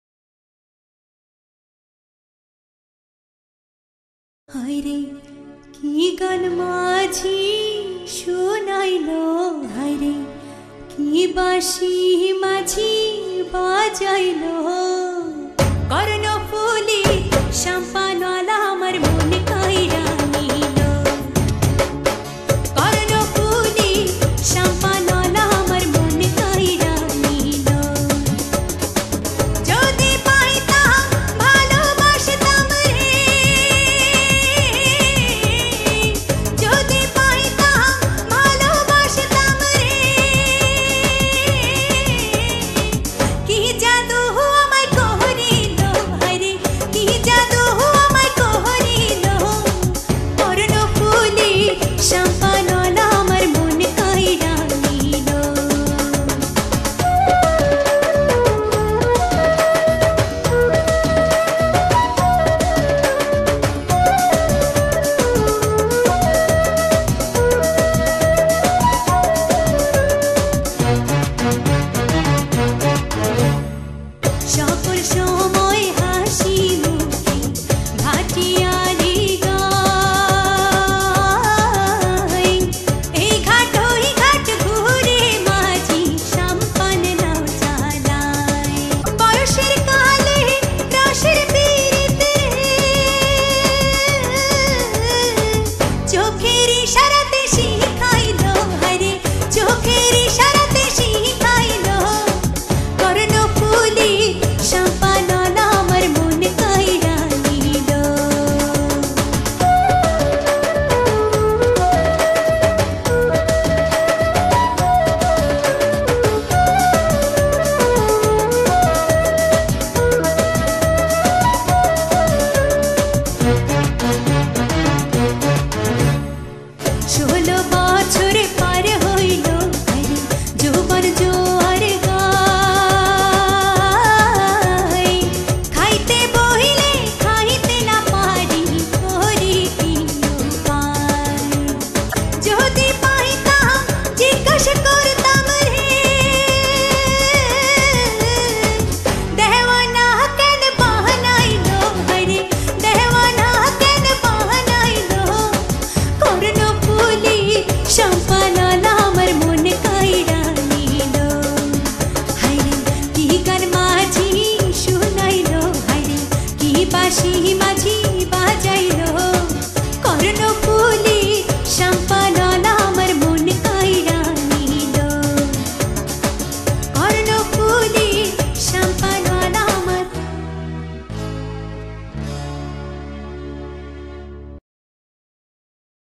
নুপুর সিজন-২এর প্রি-ফাইনাল ২এর পুরোনো দিনের সোনালি গান এর নৃত্য খ গ্রুপ
নুপুর নৃত্য প্রতিযোগিতা সিজন -২ (নুপুর) 'এর প্রি-ফাইনাল রাউন্ড-২ এর  ' পুরনো দিনের সোনালি গান' এর নৃত্যের ভিডিও পর্ব -১৫